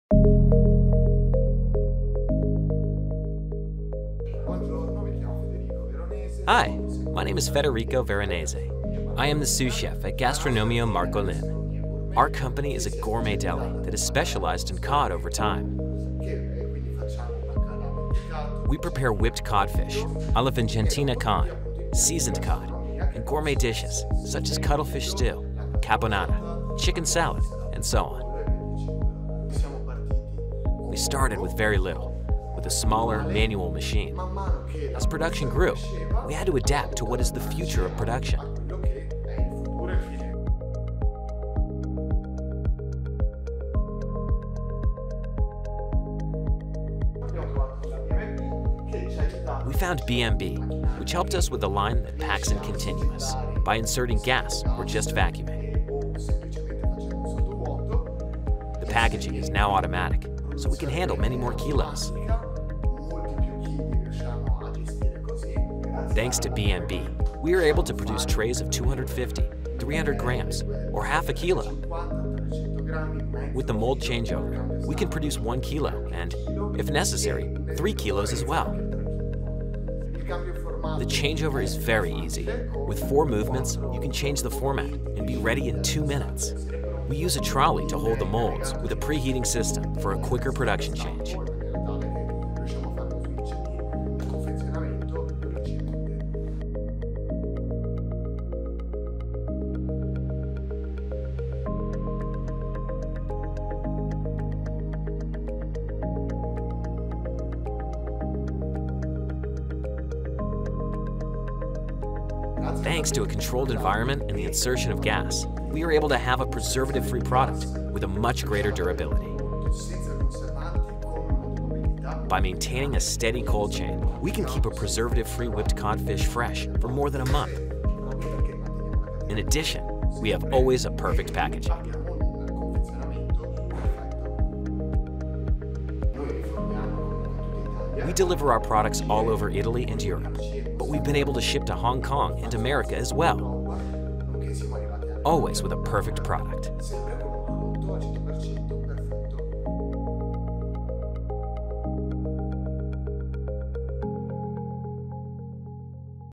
BMB - Our TSC 70 line for Gastronomia Marcolin
Meet Federico Veronese, Sous-Chef at Gastronomia Marcolin, a gourmet deli that specialised in cod. He tells us about how the company had to adapt to the change in production, which led to choosing the BMB’s TSC 70 for the packaging. Discover the advantages of vacuum sealing or modified atmosphere sealing!